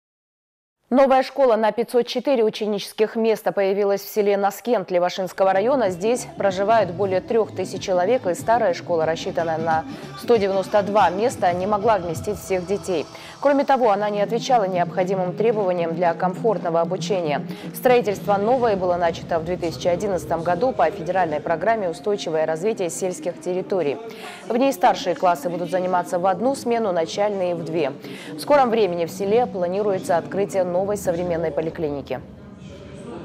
В Левашинском районе Дагестана открыли новую школу
Новая школа на 504 ученических места появилась в селе Наскент Левашинского района. Здесь проживают более трех тысяч человек, и старая школа, рассчитанная на 192 места, не могла вместить всех детей. Кроме того она не отвечала необходимым требованиям для комфортного обучения.